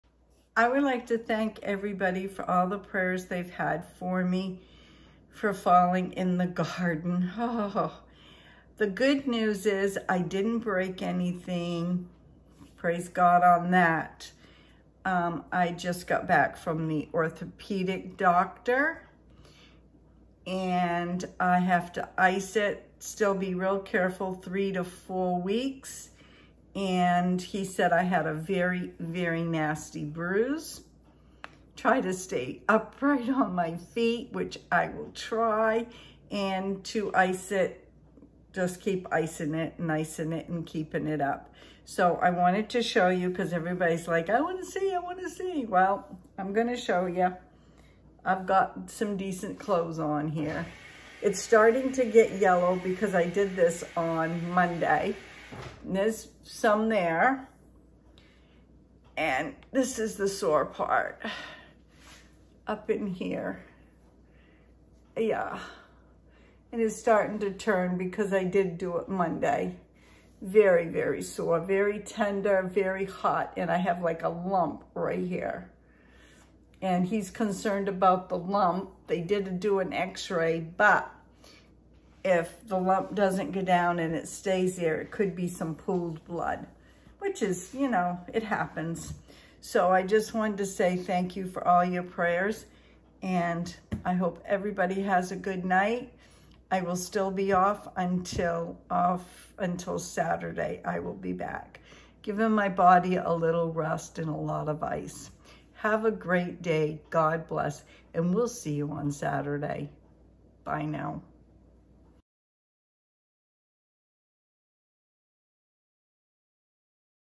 October 5, 2023 Medical up date on Brenda
Medical update on Brenda  @MAINEiacGrammiesHomestead  I fell in the garden face first again . I feel on Monday I saw my Dr and she wanted a follow up with Orthopedic I had an appointment today wow it was quick I had an X-ray and good news nothing broken just a bad bruise praise God ..
I will be back on Saturday ..
hugs to all that held
Me in prayer I have to  ice for the next three weeks and if I feel good I don’t need to go back to Orthopedic Dr ..God is good all the time 🙏
Brenda Wanden
72 Gordon Hill  Road
Chesterville Maine 04938